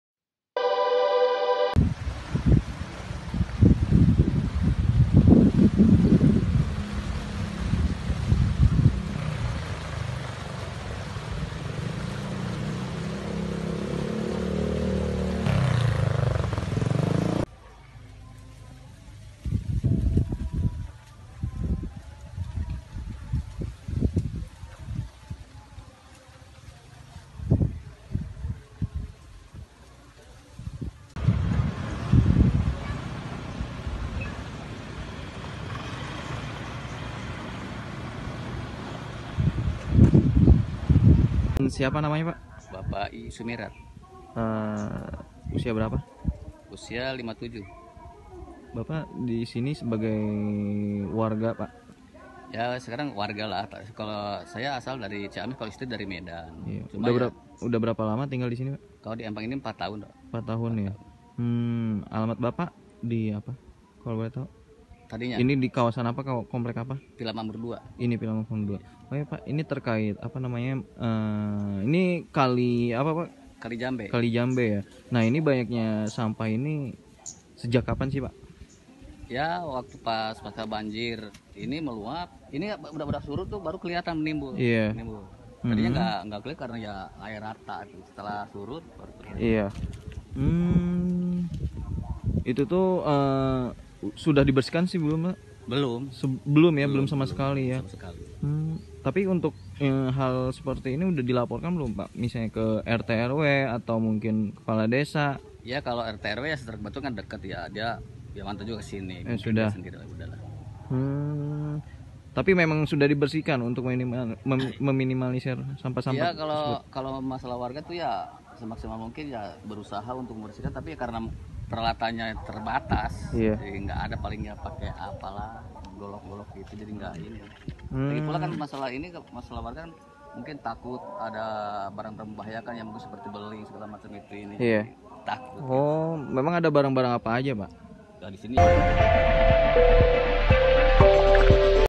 Sampah di Kali Jambe Bekasi Menumpuk dan Mengeluarkan Bau Busuk
Pemerintah Kabupaten Bekasi tak kunjung membersihkan sampah sisa banjir di Kali Jambe RW 31 Perumahan Vila Makmur Dua, Desa Mangunjaya, Kecamatan Tambun Selatan, Kabupaten Bekasi, Jawa Barat.

Pantauan Wartakota, Jumat (17/1/2020) tumpukan sampah dengan ketinggian 1 meter dan sepanjang 100 meter masih terlihat di Kali Jambe.

Padahal tumpukan sampah ini telah ada sejak Jumat (3/1/2020) atau dua hari pascabanjir. Namun, tak kunjung dibersihkan oleh pemerintah Kabupaten Bekasi.

Nampak tumpukan ini terdiri dari sampah rumah tangga yakni kasur, lemari, pintu, dan berbagai kayu. Tak hanya itu terdapat juga sampah plastik dan styrofoam.

Selain membuat aliran air kali jambe terhenti, sampah tersebut mengeluarkan bau yang tak sedap. Banyak warga juga yang mengeluhkan kondisi ini.

Salah satu warga Perumahan Villa Makmur 2 bernama Ii Sumirat (57) mengatakan sampah dan bau menyengat ini dikhawatirkan menimbulkan berbagai penyakit.

Ia pun mengaku pemerintah Kabupaten Bekasi tak kunjung membersihkan, meski telah melaporkan kejadian ini ke pejabat daerah setempat.

"Belum ada yang memantau dari pemerintah Kabupaten Bekasi jadi makanya sampah kaya dibiarkan saja," kata dia kepada Wartakota saat ditemui di lokasi, Jumat (17/1/2020).

Menurutnya, terkadang warga Perumahan Vila Makmur 2 berinisiatif melakukan pembersihan sendiri secara manual menggunakan peralatan seadanya.

"Iya kita kerja bakti aja, paling mungkin baru 10 persen yang baru terangkut sampah sama warga karena kekurangan alat juga kan," tegas dia.

Dirinya berharap agar pemerintah Kabupaten Bekasi segera turun tangan untuk mengangkut sampah ini supaya tidak menumpuk lebih banyak lagi dan membuat saluran kali jambe berangsur membaik.(M20)